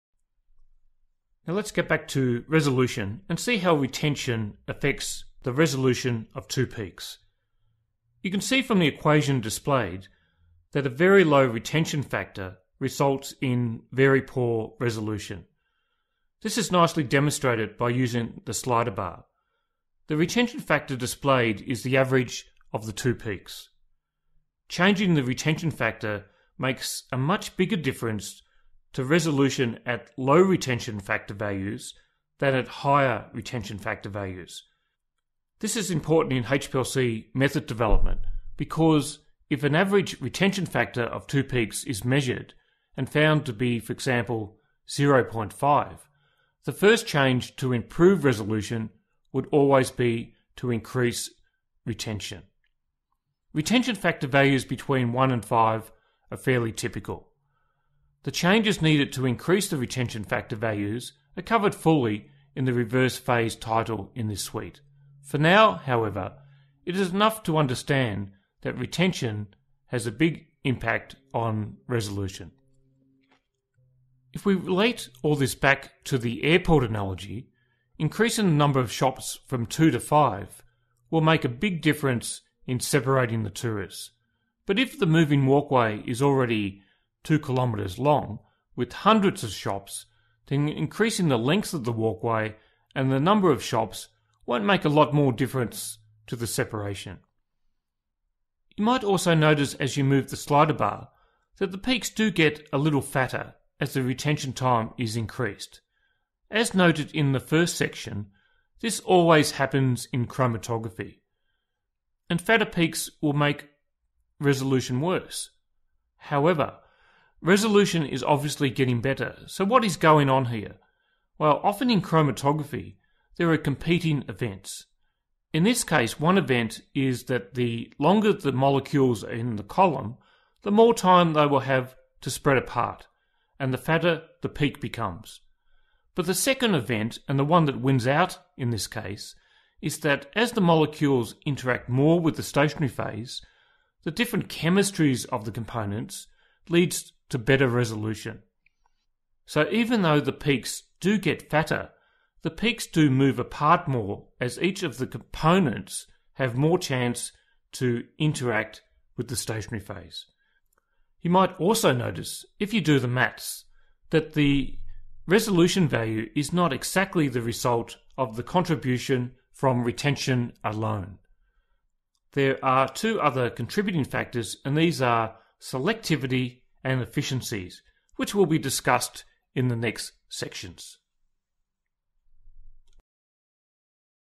Fundamentals of HPLC 11 - Retention and Resolution
This video describes the relationship between retention and resolution using the retention factor (capacity factor).